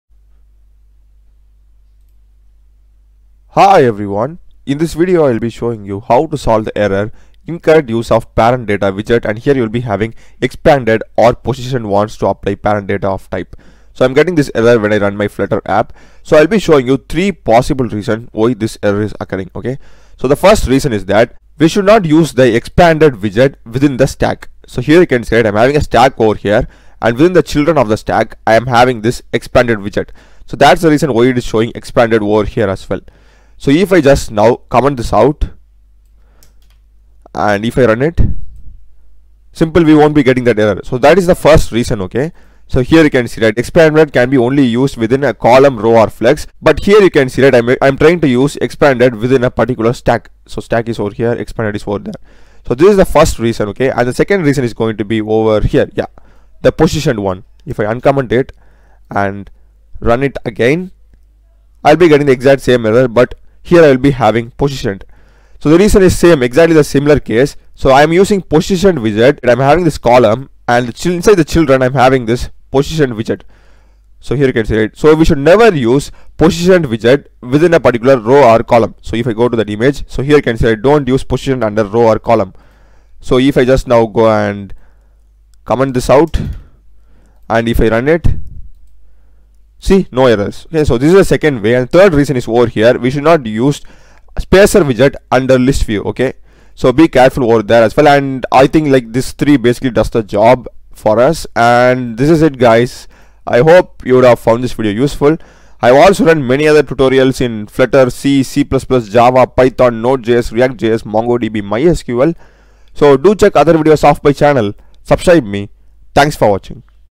Incorrect use of parentdatawidget in flutter SOLVED
How to fix the error Incorrect use of parentdatawidget , expanded and positioned wants to apply parentdata in flutter is shown